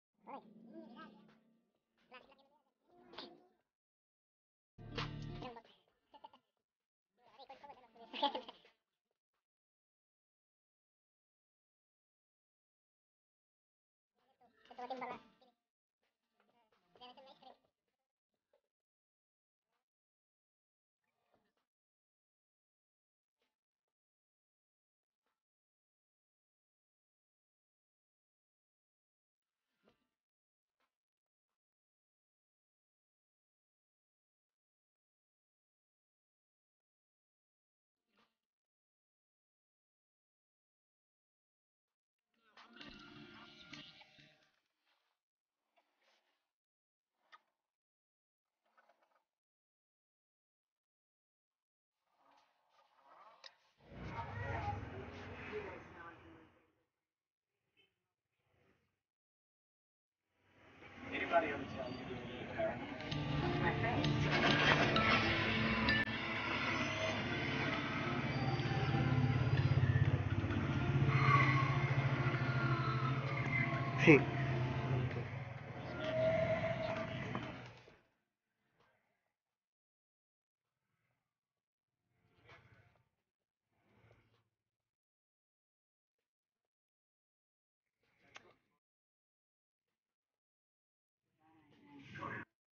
Tie Dye Tshirt - Dark Rainbow Spiral
Ishare ko lang po ang bagong dye na nabili ko. Sulit at legit!

Shout out ky Denver (siya ung nasa unahang bahagi ng video).